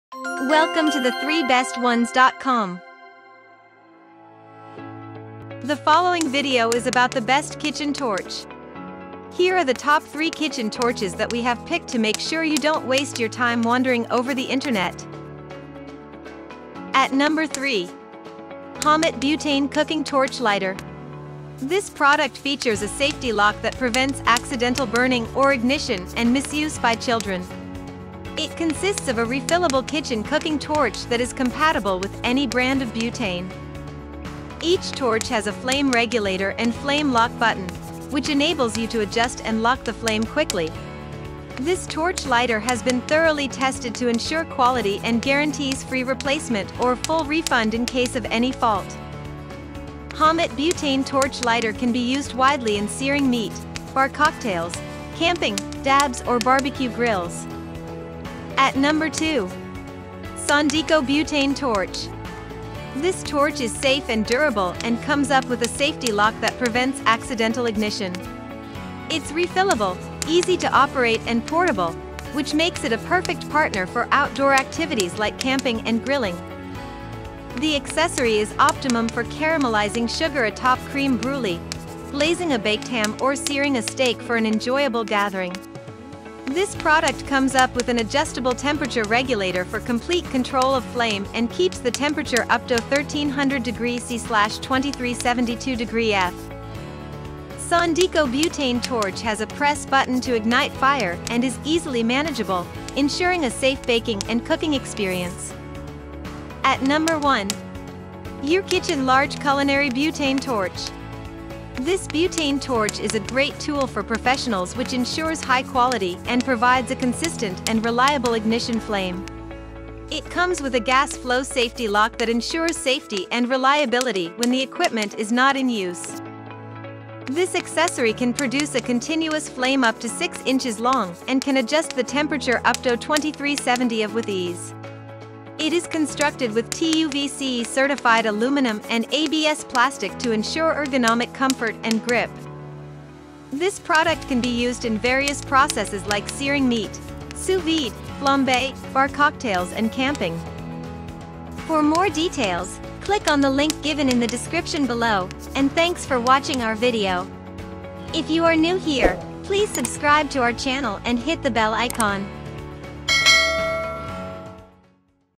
Best Kitchen Torches In 2023
Follow the links given below to check the product prices:

Here is the list of the top 3 best "Kitchen Torches":

EurKitchen Large Culinary Butane Torch:

Sondiko Butane Torch:

HOMMIT Butane Cooking Torch Lighter:

0:00 The Best Kitchen Torches Reviewed
0:16 HOMMIT Butane Cooking Torch Lighter
0:58 Sondiko Butane Torch
01:46 EurKitchen Large Culinary Butane Torch
02:31 Closing Remarks & Subscribe To Our Channel